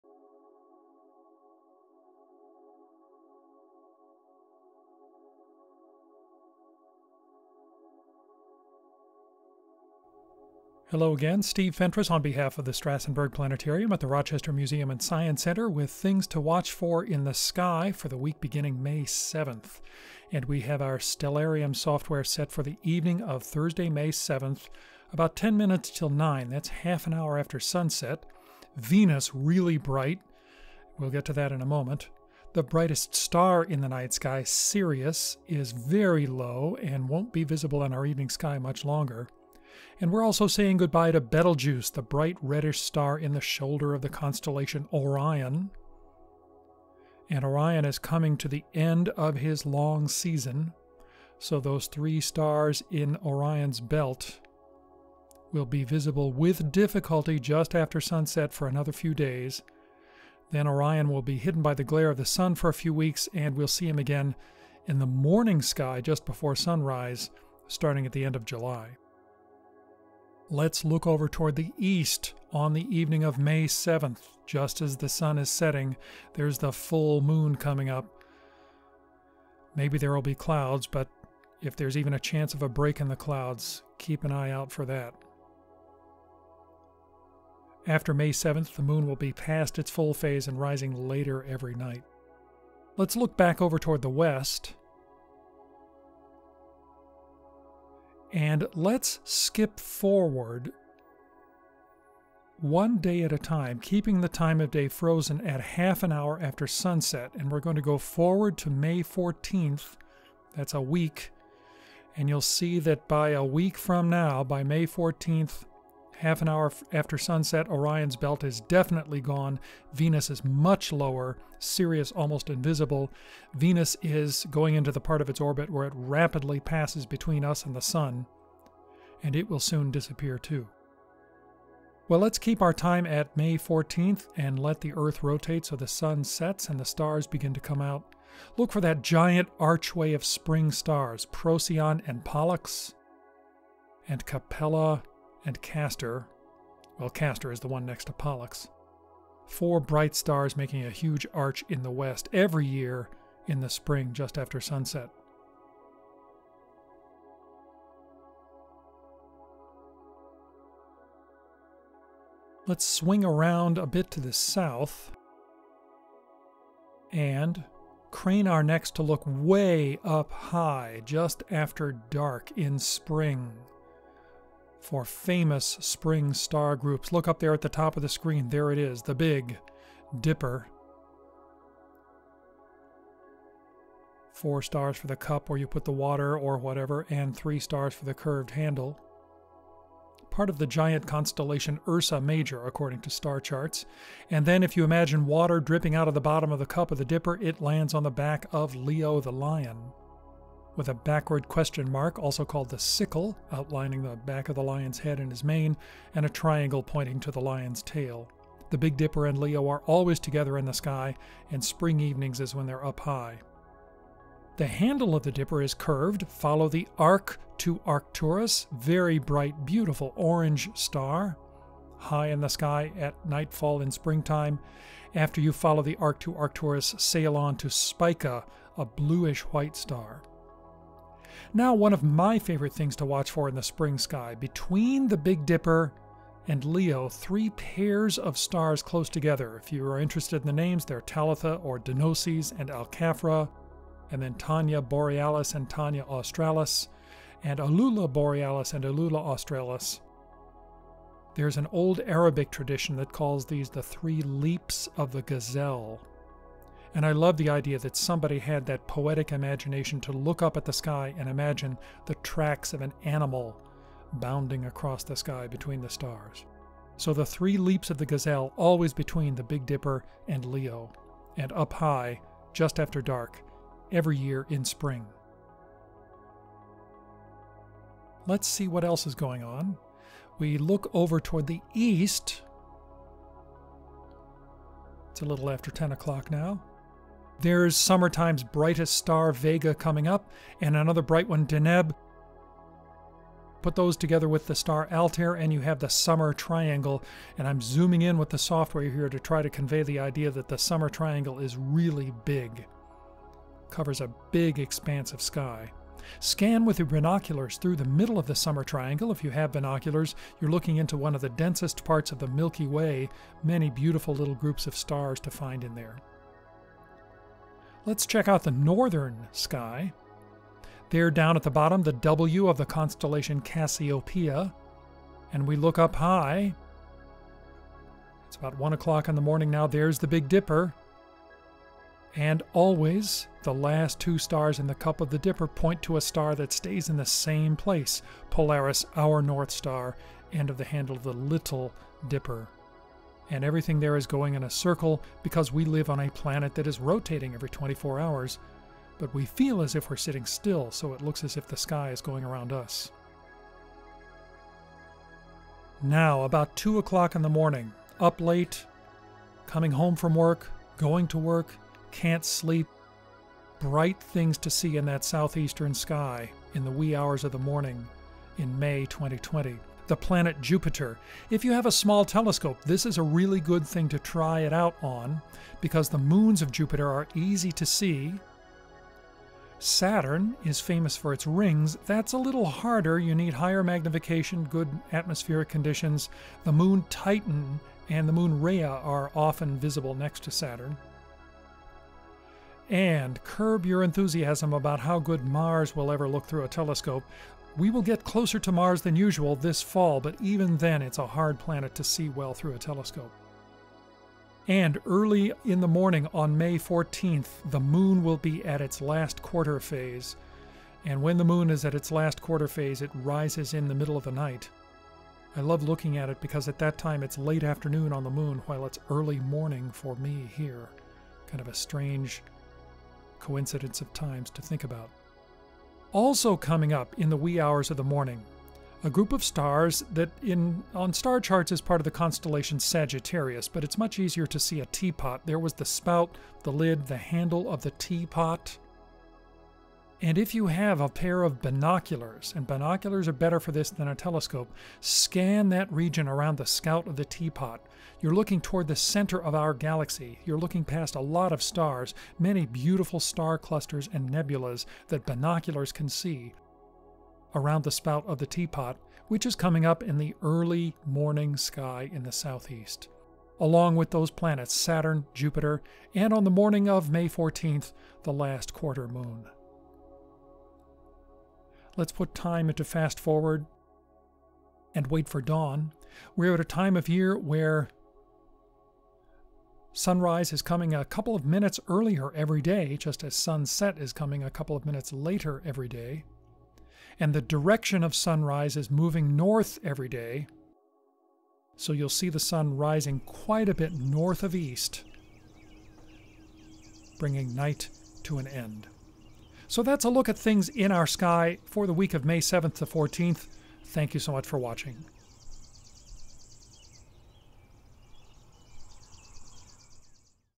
Virtual Planetarium: The Sky this Week 5/7
Who’s ready for some stargazing? Explore “The Sky This Week” with our Planetarium Director, Steve, to learn what constellations, star clusters, and planets you can see in the night sky.